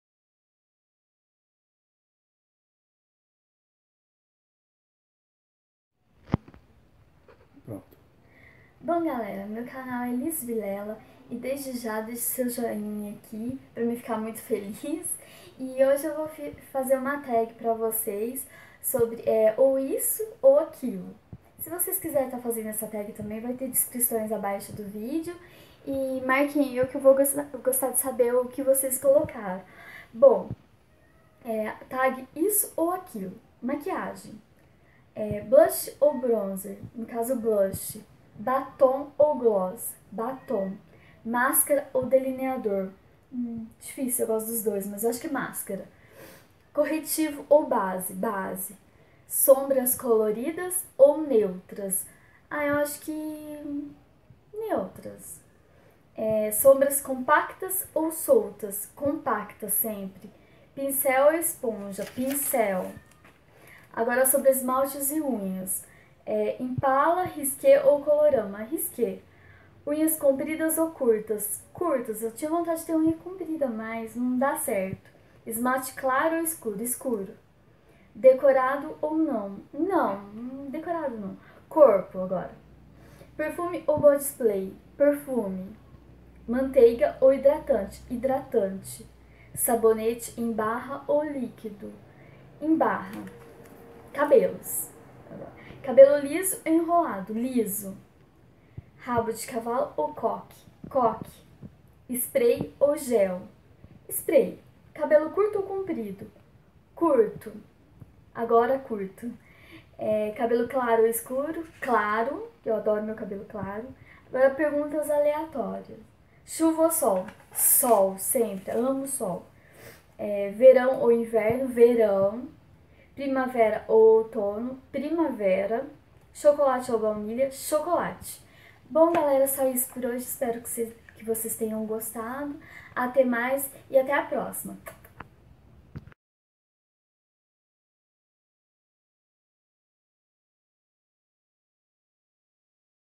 Tag Isso ou Aquilo
Uma tag super rapidinha pra vcs me conhecerem mais :) ♥
Perguntas – Isso ou Aquilo?

MAQUIAGEM
Blush ou Bronzer?
Batom ou gloss?
Máscara ou Delineador?
Corretivo ou Base?
Sombras coloridas ou sombras neutras?
Sombras compactas ou soltas?
Pincél ou esponja?
ESMALTE/UNHAS
Impala, Risque ou Colorama?
Unhas compridas ou curtas?
Esmaltes claros ou escuros?
Com ou sem florzinha?
CORPO
Perfume ou body splash?
Manteiga ou hidratante?
Sabonete em barra ou líquido?
CABELOS
Cabelos lisos ou enrolados?
Rabo de cavalo ou coque?
Spray ou gel?
Cabelo comprido ou curto?
Cabelo claro ou escuro?
PERGUNTAS ALEATÓRIAS
Chuva ou sol?
Verão ou inverno?
Primavera ou outono?
Chocolate ou baunilha?